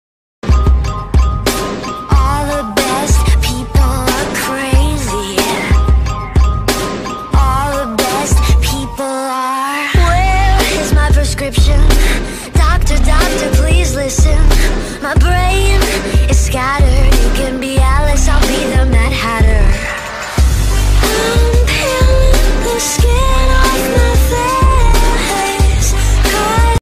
Mad Hatter❤️🖤
⚠️TW: Fake Skin⚠️Mad hatter is one of my favorite Johnny Depp😆 watch me create his look❤️🖤